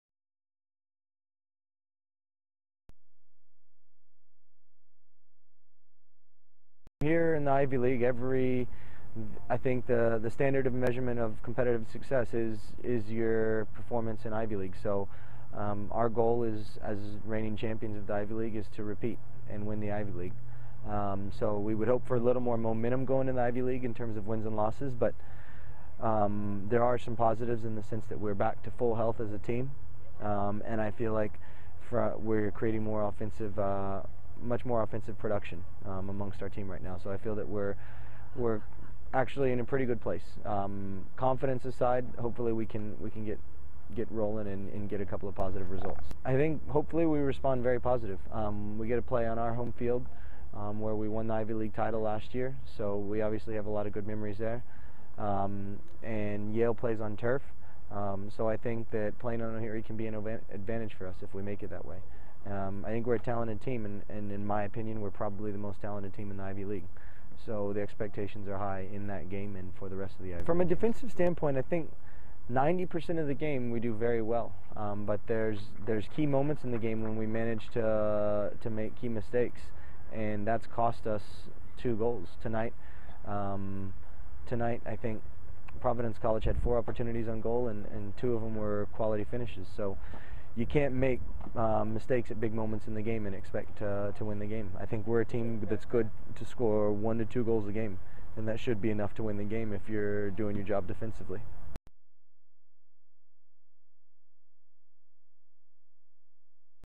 Men's Soccer vs. Yale Preview
The Harvard men's soccer team opens its Ivy League schedule by hosting archrival Yale at Ohiri Field Saturday, Oct. 2.